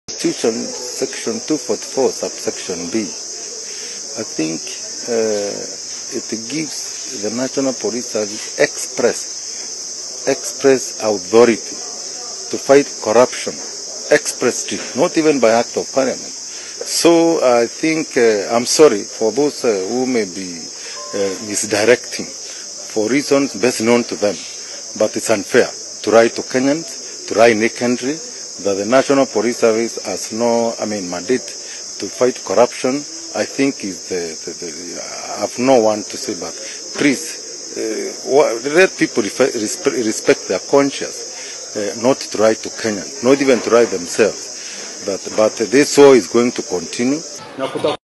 NPS HAVE EXPRESS POWERS TO INVESTIGATE CORRUPTION , DCI KINOTI TELLS OFF POLITICIANS.
Director of Criminal Investigation George Kinoti has responded to deputy president william Ruto allies who claimed DCI has no powers to investigate corruption.